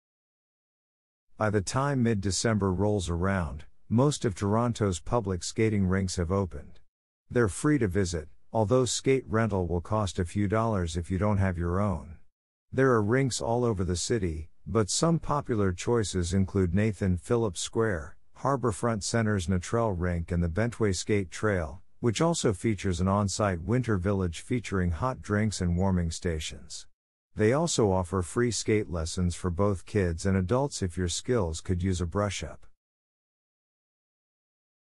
Do Some Skating
By the time mid-December rolls around, most of Toronto's public skating rinks have opened. They're free to visit (although skate rental will cost a few dollars if you don't have your own). There are rinks all over the city, but some popular choices include Nathan Philips Square, Harbourfront Centre's Natrel Rink and The Bentway Skate Trail, which also features an on-site winter village featuring hot drinks and warming stations. They also offer free skate lessons for both kids and adults if your skills could use a brush-up..